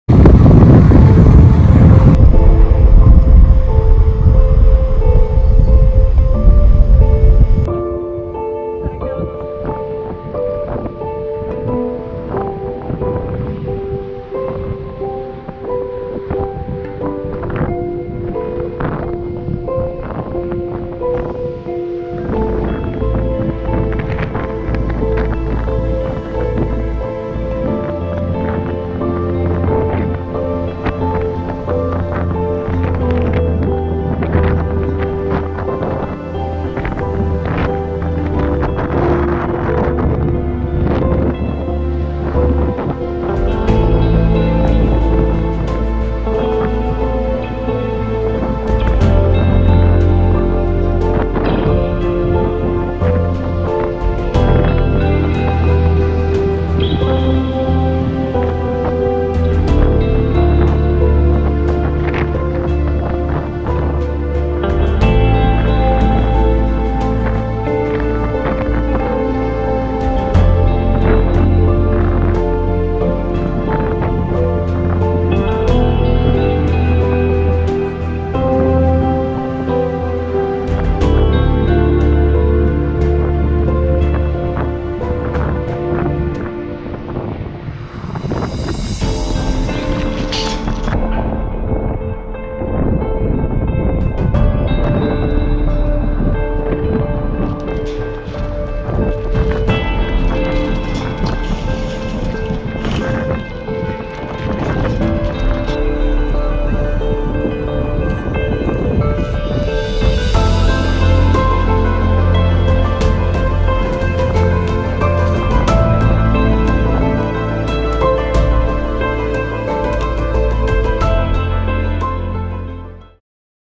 LIVING ON THE SIDE OF THE MOTORWAY!
Motorway recsues are always horrible as one mistake can be the difference bewteen life and death. This podenca is extremely smart and was suspicious of the trap but we had a plan and this is what happened next... Meet Lisa!

Los reclamos en las autopistas son siempre horribles, ya que un error puede marcar la diferencia entre la vida y la muerte. Esta podenca es extremadamente inteligente y sospechaba de la trampa, pero teníamos un plan y esto es lo que pasó después ... ¡Conoce a Lisa!